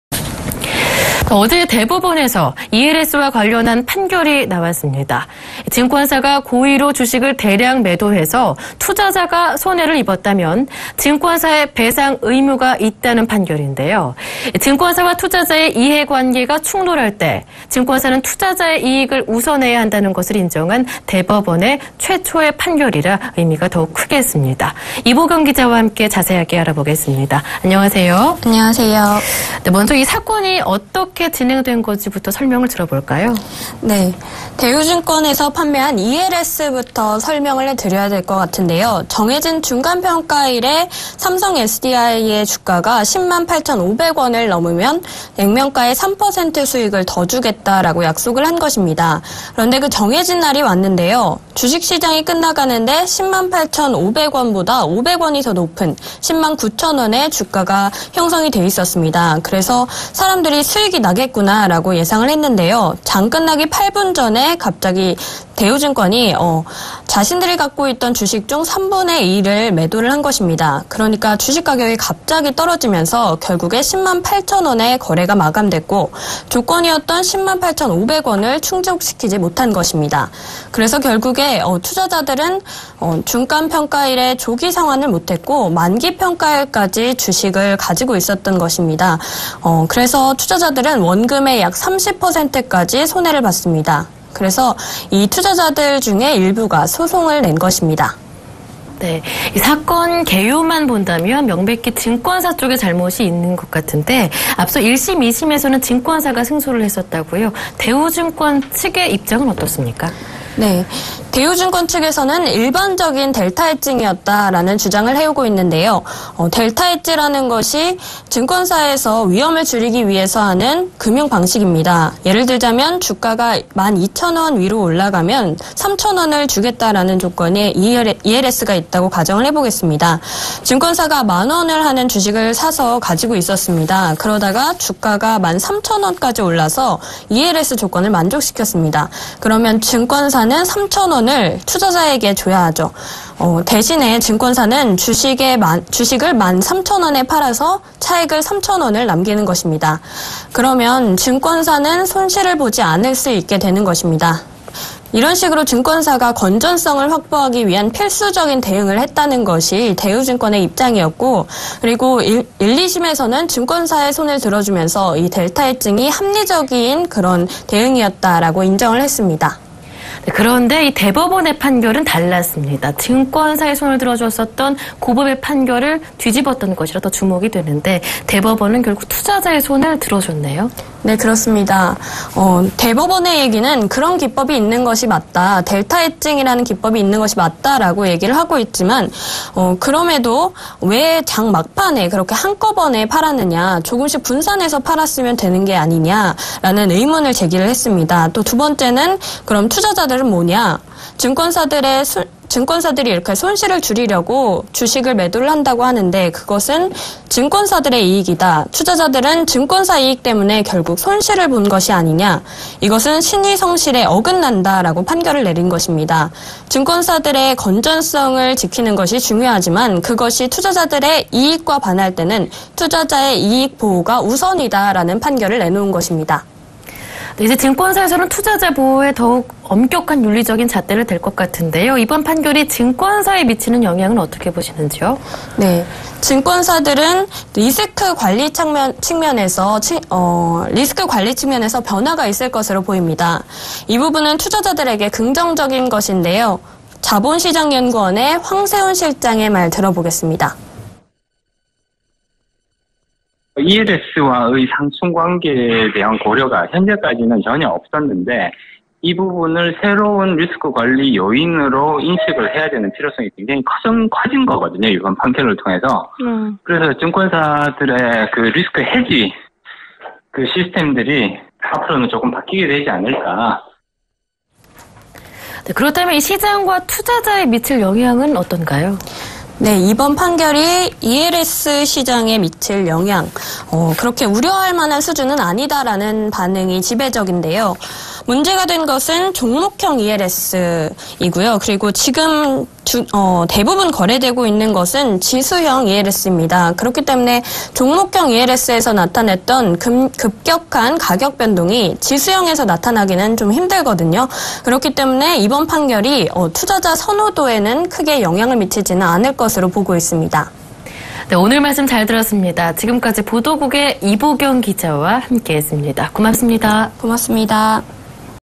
[투데이 포커스] ‘투자자 이익 우선’ ELS 판결
[앵커]
어제 대법원에서 ELS와 관련한 판결이 나왔습니다. ‘증권사가 고의로 주식을 대량매도해서 투자자가 손해를 입었다면 증권사에 배상 의무가 있다’라는 판결인데요. 증권사와 투자자의 이해관계가 충돌할 때 증권사는 투자자의 이익을 우선해야 한다는 것을 인정한 최초의 판결이라 의미가 큽니다. 이보경기자와 자세히 알아보겠습니다.

[앵커]
사건이 어떻게 진행된건지 설명해주시죠

[기자]
대우증권에서 판매했다는 ELS부터 설명을 해야 할꺼 같은데요
“정해진 중간평가일에 삼성SDI 주가가 10만 8,500원을 넘으면 액면가의 3% 를 더 주겠다”라고 약속을 한 거였습니다. 그런데 그 정해진 날이 왔는데요. 주식시장이 끝나가는데 10만8,500원보다 500원이 높은 10만 9,000원에 주가가 형성돼 있었습니다. 그래서 사람들이 수익이 나겠구나라고 예상을 했는데요. 장 끝나기 8분 전에 갑자기 대우증권이 10만8,000 원에 자신들이 갖고 있던 주식 중에 3분의 1을 매도한 것입니다.
그러니까 주식가격이 갑자기 떨어지면서 결국  10만8,000원에 거래가 마감됐고 조건이었던 10만 8,500원을 충족시키지 못한거죠.
그래서 결국 투자자들은 중간평가일에 조기상환을 못했고 만기평가일까지 주식을 가지고 있던 투자자들이 원금의 약 30%까지 손해를 봤습니다, 그래서 이 투자자 중에 일부가 소송을 낸 것입니다.

[앵커]
사건 개요만 듣기에는 증권사측에 명백한 잘못이 있는 것 같은데…1,2심에서는 승소를 했었다고요? 대우증권측의 입장이 어땠나요?

[기자]
대우증권측에서는 일반적인 ‘델타헷징’을 한 것이다라는 주장을 해왔습니다.
‘델타헷지’라는 것은 증권사에서 위험을 줄이기 위해서 사용하는 금융방식인데요.
쉽게 말해서, 주가가 1만2,000원 위로 올라가면 3,000원을 주겠다라는 조건의 ELS가 있다고 가정해보겠습니다.
증권사가 1만원하는 주식을 사서 가지고 있어요. 그러다가 주가가 1만3,000원까지 올라서 조건을 만족시켰어요. 그러면 증권사는 3,000원을 투자자에게 줘야 하죠. 대신 증권사는 주식을 1만3,000원에 팔아서 차익을 3,000원을 남기는겁니다. 그럼 증권사는 손실을 보지않을 수 있게 되는겁니다.
 이런 식으로 증권사 건전성을 확보하기 위한 필수적인 대응을 한 것이다라는 것이 대우증권의 입장이었고 그리고 1,2심에서는 증권사의 손을 들어주면서 이 델타헷징이 합리적인 대응이었다고 인정해줬던거죠.

[앵커]
대법원의 판결은 달랐죠? 대우증권이 아닌 투자자의 손을 들어줬는데요.

[기자]
대법원 얘기는 “그런 기법이 있는 것 맞다. 맞는데, 그렇다고 하더라도 그럼 왜 장 막판에 그렇게 한꺼번에 팔았느냐, 조금씩 분산해서 팔았으면 되는 것 아니냐?” 그리고 또 두번째는 “그럼 투자자들은 뭐냐? 증권사들이 손실을 줄이려고 그렇게 주식을 매도를 한다고 하는데 그것은 증권사의 이익이다. 투자자들은 증권사 이익 때문에 결국 손실을 본 것이 아니냐. 이것은 신의성실에 어긋난다“라는 판결을 내린 것입니다.
증권사의 건전성을 지키는 것이 중요하지만 그것이 투자자의 이익과 반할 때는 투자자의 이익 보호가 우선이다라고 못을 박은거죠.

[앵커]
이번판결이 증권사에 어떤 영향을 미쳤나요?

[기자]
증권사들의 리스크 관리 측면에서 변화가 있을 것으로 보이는데요. 이 부분은 투자자들에게 긍정적인 것이죠. 자본시장연구원의 황세운 자본실장의 말 들어보시겠습니다.

[인터뷰] 황세운 실장 / 자본시장연구원
이 판결 전까지는 ELS와 리스크헷지 상충관계에 대한 고려가 지금까지 전혀 없었는데 증권사에서 이 부분을 새로운 리스크관리 요인으로 인식해야하는 필요성이 커진 것입니다. 그래서 증권사들의 리스크 헤지 시스템들이 변화될 것이라고 보입니다.

[앵커]
시장에 미칠 영향은 어떤가요? 특히 투자자들에게는요?

[기자]
이번 판결이 ELS 시장에 끼칠 영향 또한 우려할 만한 수준은 아니라는 예상이 지배적입니다.
문제가 됐던 것은 종목형 ELS고 지금 거래되고 잇는 ELS는 대부분 지수형 ELS입니다. 그렇기 때문에 종목형 ELS에서 나타났던 급격한 가격변동이 지수형에서 나타나기 힘들거든요. 그렇기 때문에 이번 판결이 투자자 선호도에는 큰 영향을 미치지는 않을 것으로 보입니다.